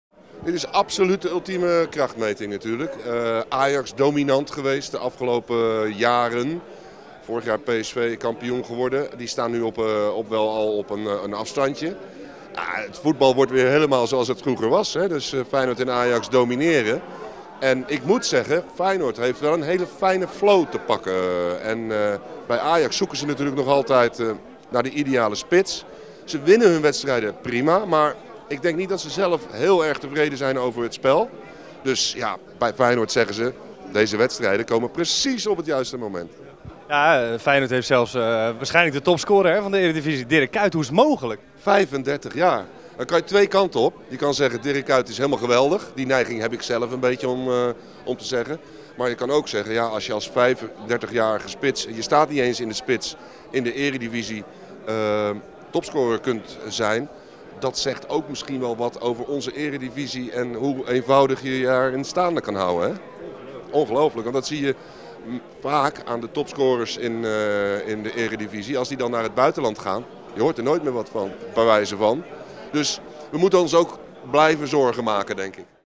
Peter van Drunen interviews 2015 - Koert Westerman over Feyenoord met Kuijt
Koert Westerman spreekt met journalist Peter van Drunen over de titelkansen van Feyenoord onder aanvoering van Dirk Kuijt.